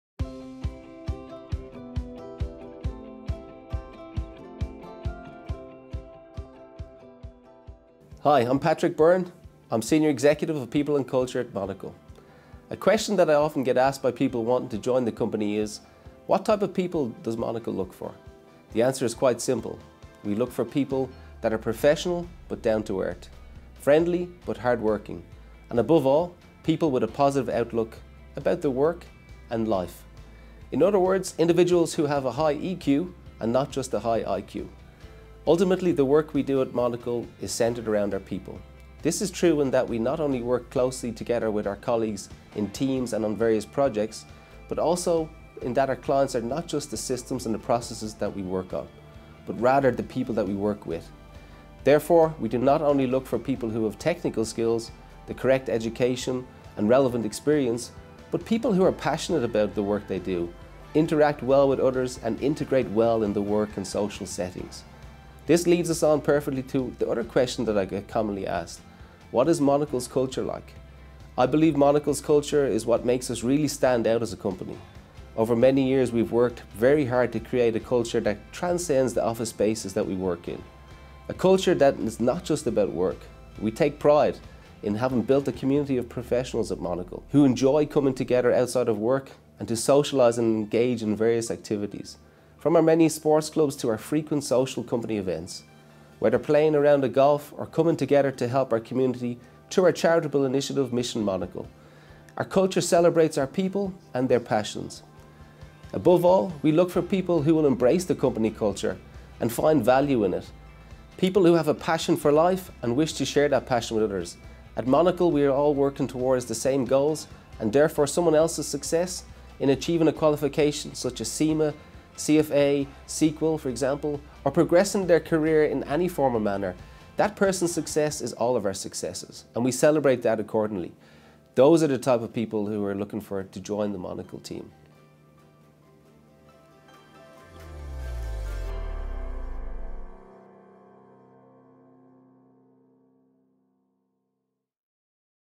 The people of Monocle
Patrick Byrne, Senior Executive of People & Culture at Monocle, talks about the sort of people we hire at Monocle and why culture is so important.